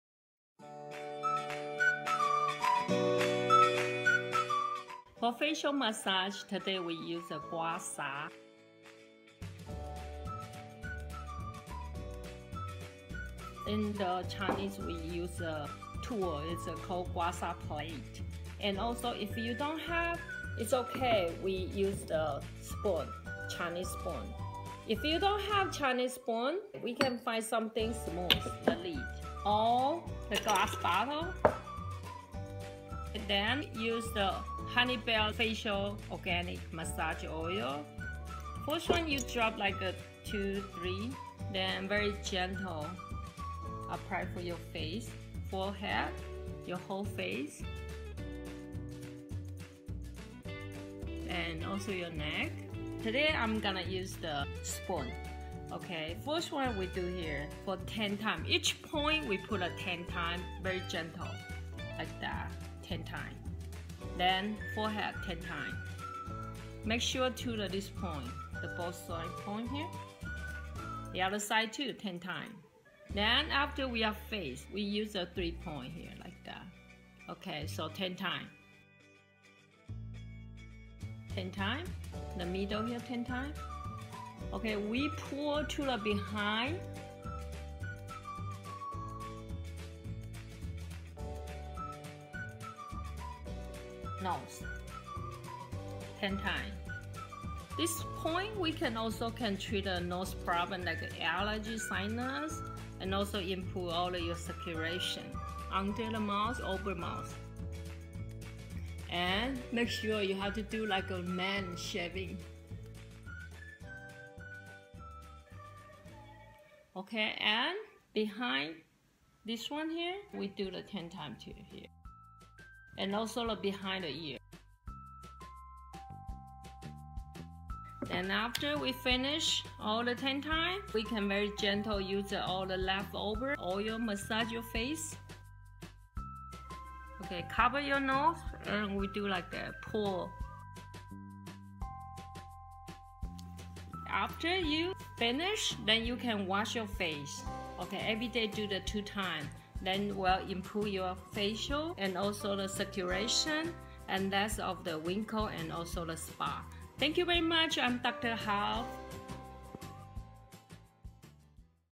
Gua Sha - Facial Massage for Anti-Aging w/ Dr. Tina Hou
Gua Sha is a traditional Chinese medical treatment that releases unhealthy elements from areas and stimulates blood flow and healing to treat wrinkles, age spots, and circulation and allergy problems. For added antioxidant and anti-aging power, use an organic facial oil packed with vitamins and antioxidants.

The one used in this video is one I highly recommend! Honey Belle's ELIXIR organic facial oil is made out of organic cold pressed hemp seed, and it contains healthy fats to repair the skin, heal wrinkles, and promote youth.
By Dr. Tina Hou（侯淑紅針灸中醫師）